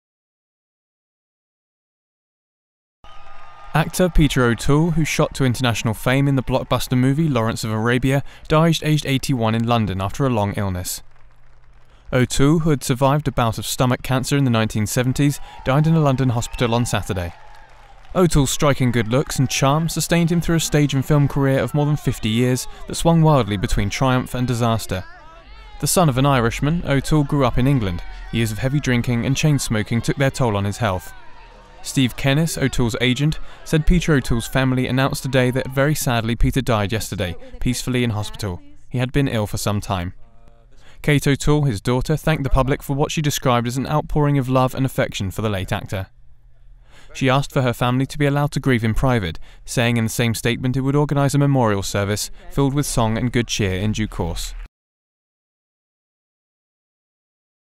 Actor Peter O'Toole Dies in London Aged 81
Actor Peter O'Toole, who shot to international fame in the blockbuster movie "Lawrence of Arabia" died aged 81 in London after a long illness, his agent said on Sunday (December 15).